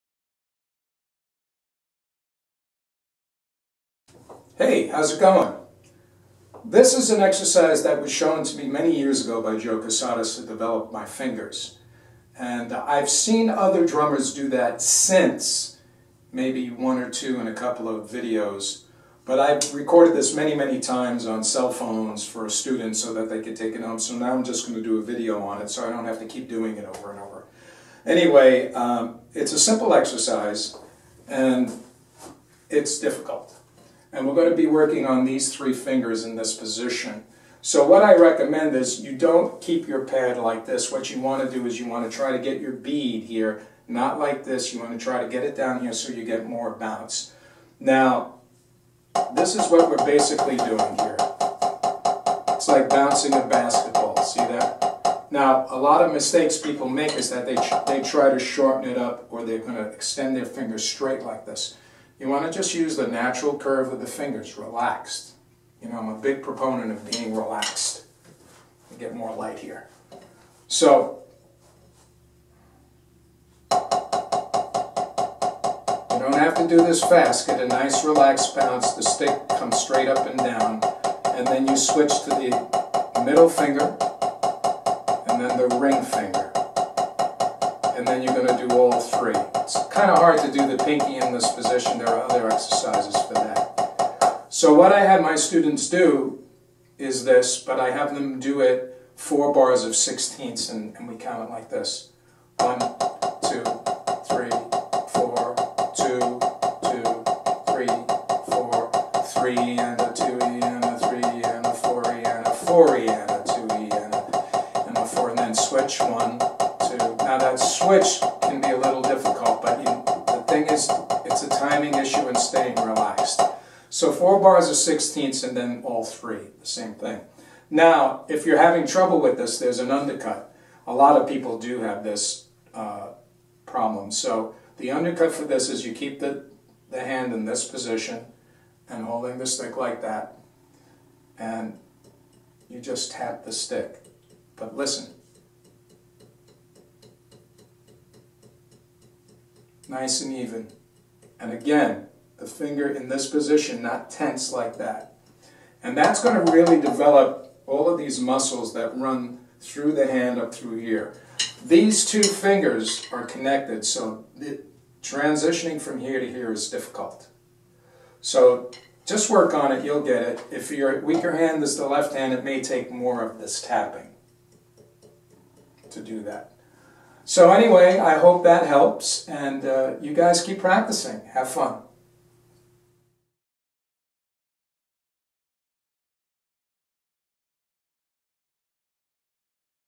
Developing Fingers for Drumming
This exercise was shown to me many years ago by Joe Cusatis. The concentration is on the index, middle and ring fingers. The pinky is address in a different exercise. It is important to turn the pad so that the angle is pointing a way from you for a better bounce and you rest your elbow on your knee so as to only move your fingers and relax.